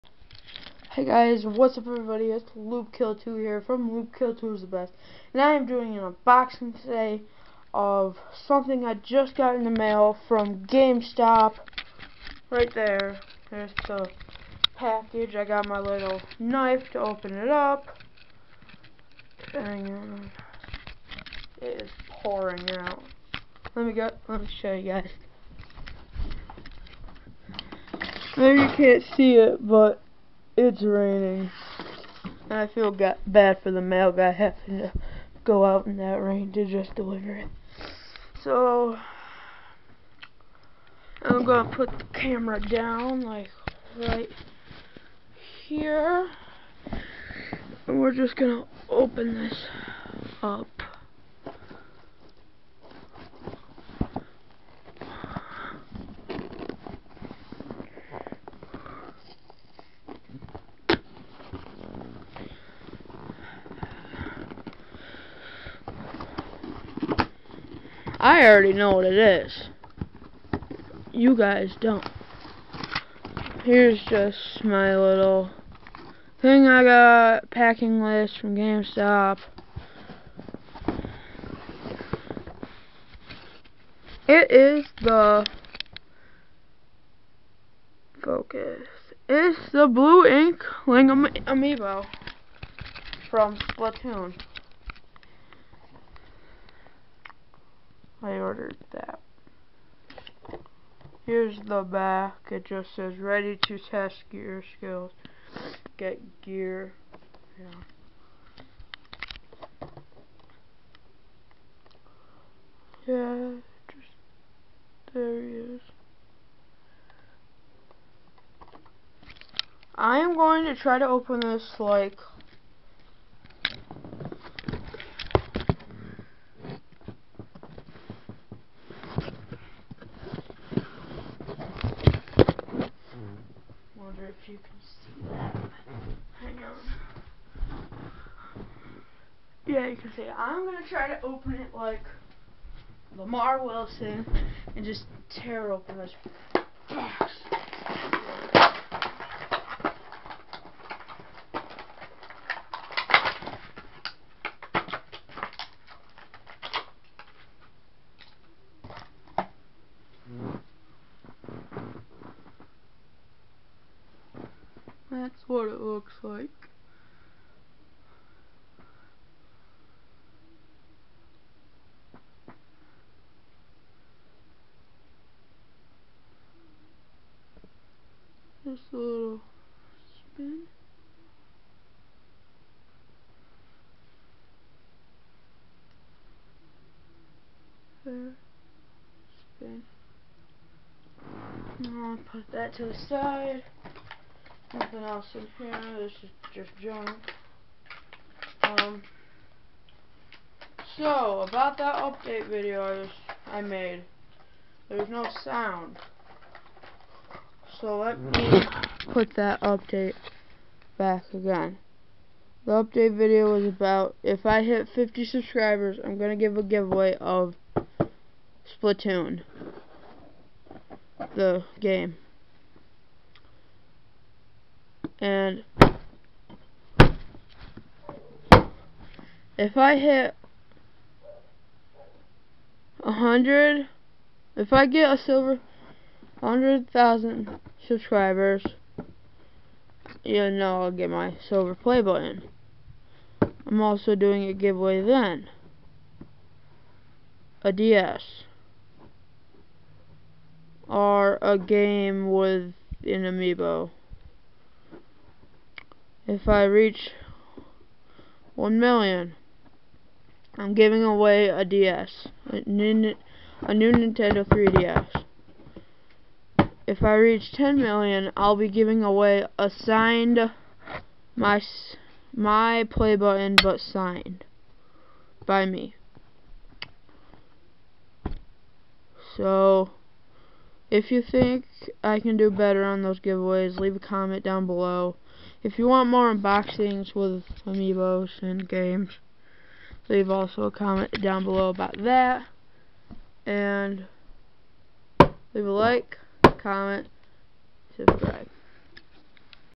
boy inkling unboxing plus update
Loopkill2's Streaming/Recording Gear. today i unbox the boy inkling amiibo